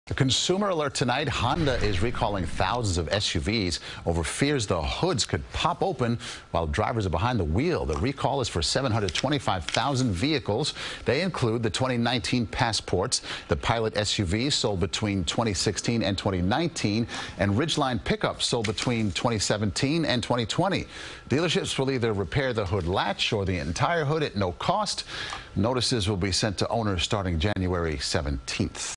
Honda Recalls SUVs Over Fears Hoods Could Pop Open While Driving
Honda is recalling hundreds of thousands of SUVs over fears hoods could pop open while drivers are behind the wheel.